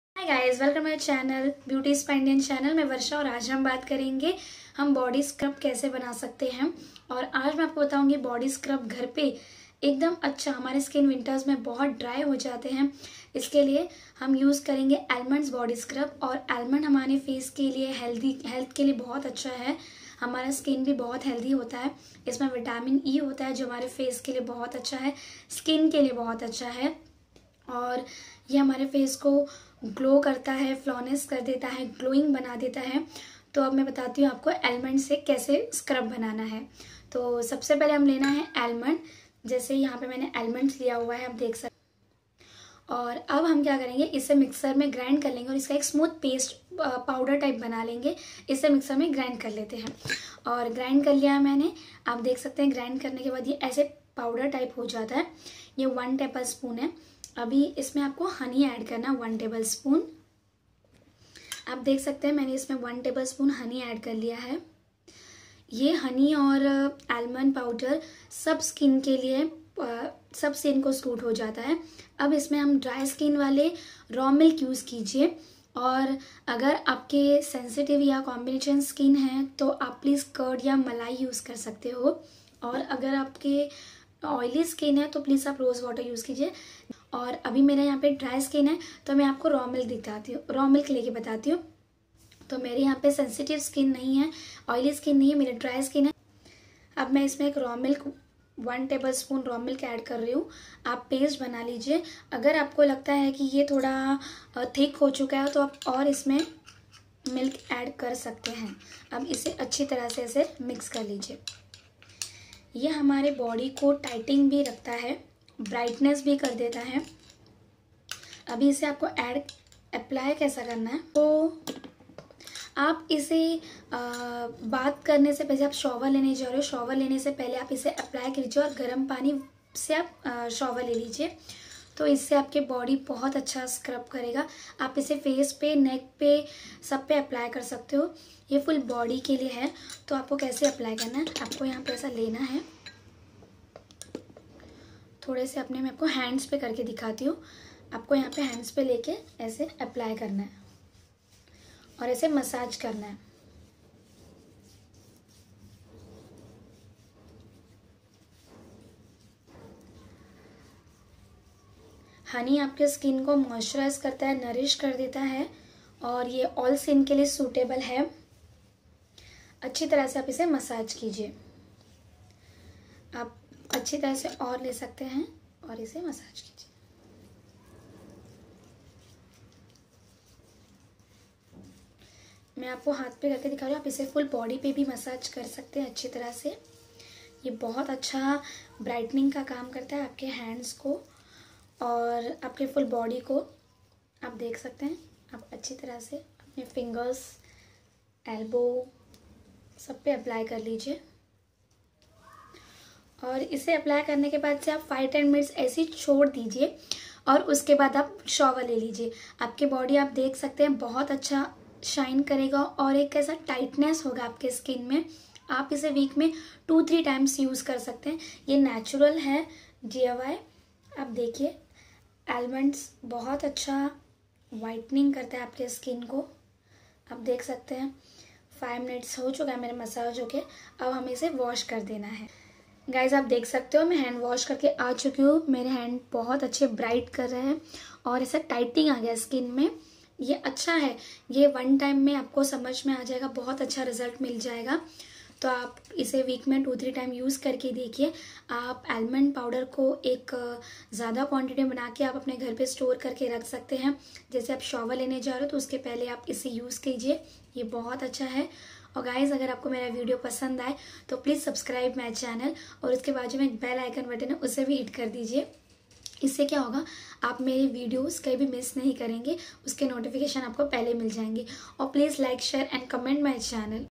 DIY how to do full body scrub at home
Diy of body scrub
Almond body scrub for 3types of skin
Dry skin use almond powder honey and rawmilk
Combination skin use almond powder honey and curd or mlaai
Oily skin use almond powder honey and rosewater